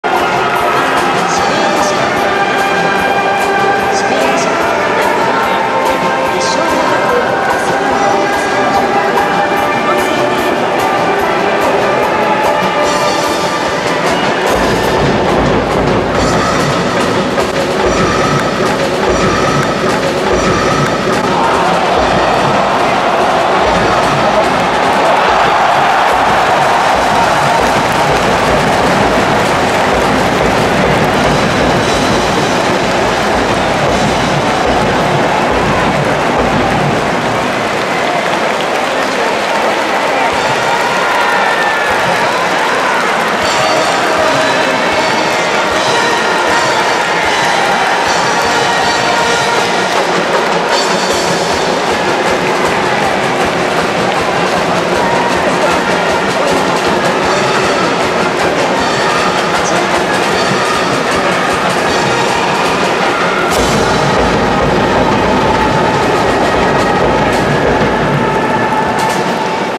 【先制ホームラン】皆川仁（大阪ガス）小谷野投手から先制ホームランを放つ
初球を振り抜き先制ホームラン